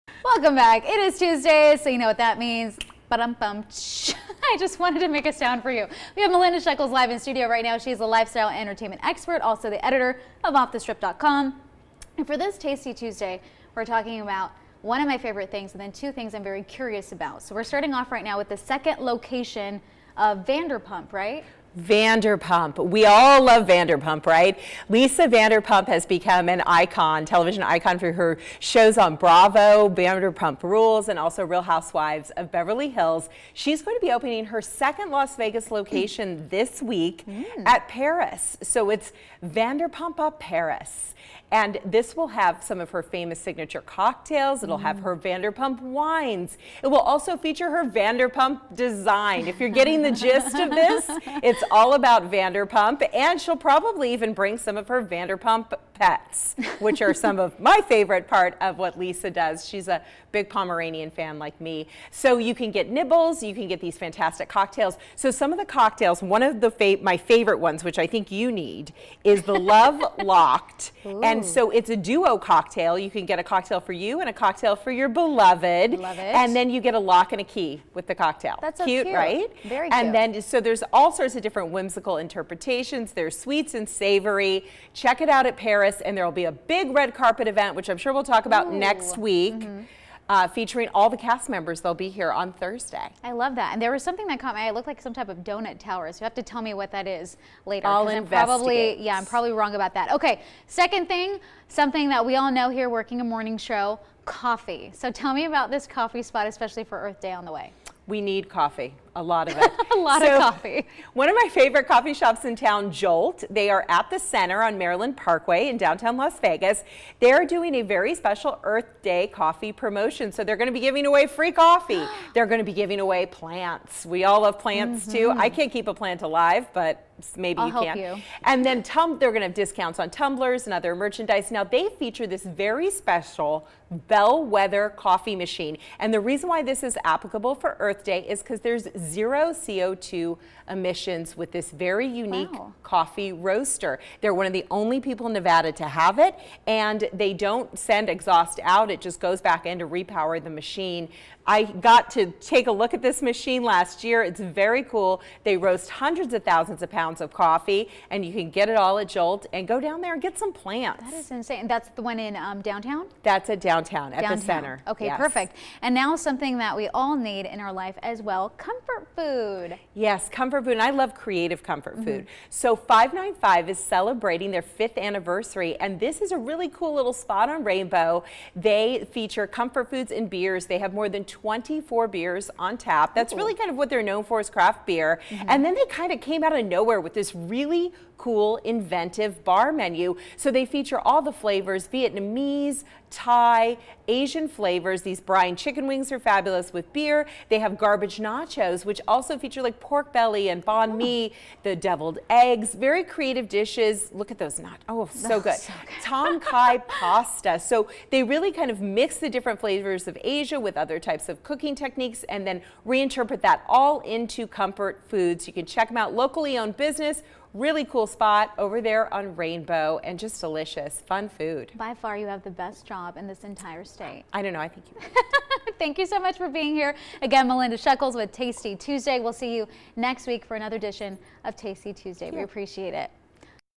Tasty Tuesday
KTNV  shares information about food spots in the LAs Vegas area.